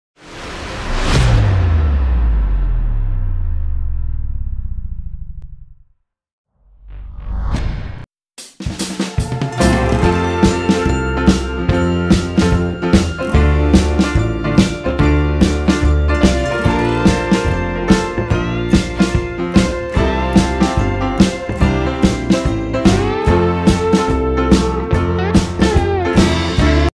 Gopro Hero 7 Black test linear vs wide & superview
Gopro Hero 7 Black test 當汽車行車紀錄器用時
在FOV Linear, Wide & Superview的視角表現